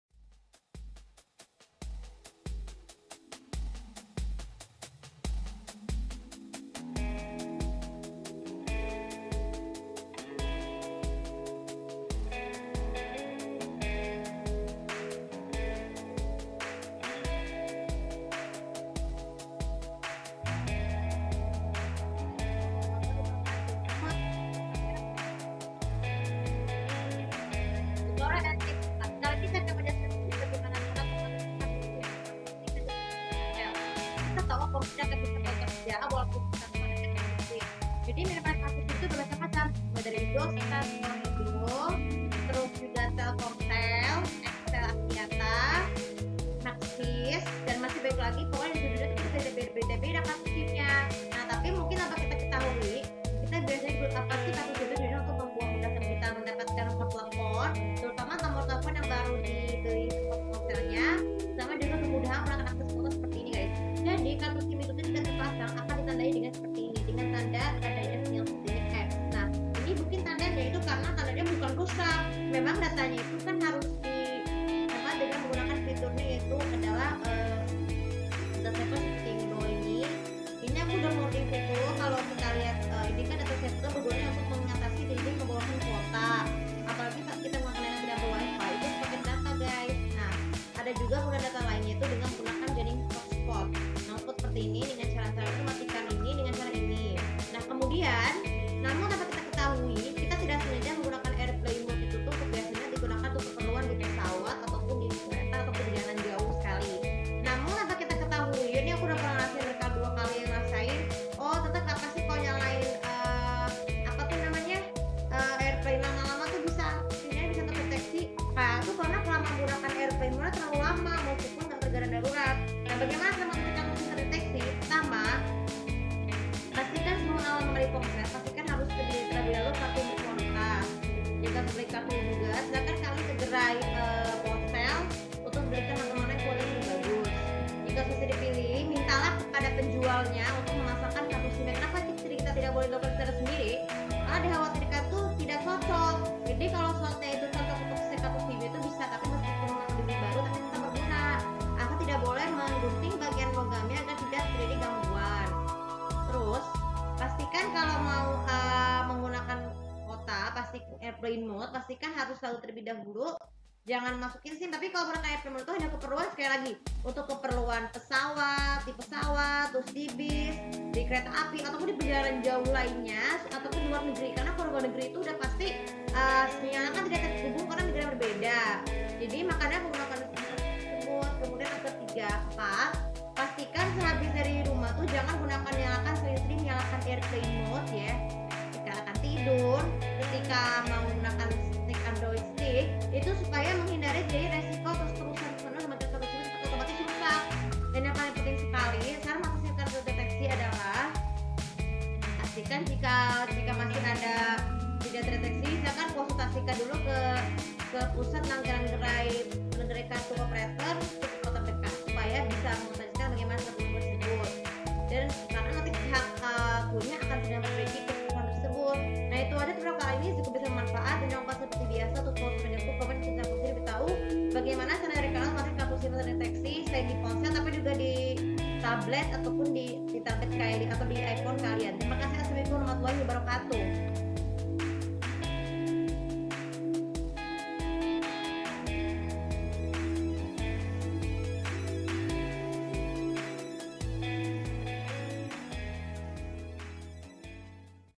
Tutorial & Tips 4 Episode 51 - Cara Mengatasi SIM Card Tidak Terdeteksi Di HP Android
Video ini saya akan menunjukkan bagaimana cara untuk mengatasi kartu SIM yang tidak terdeteksi di HP android meski sudah terpasang yang mengakibatkan kehilangan jaringan sinyal dan kesulitan untuk berkomunikasi.

Follow sosial mediaku:
Instagram & Threads : Saskia Rafifatunnisa (@rafifasaskia) (Follow)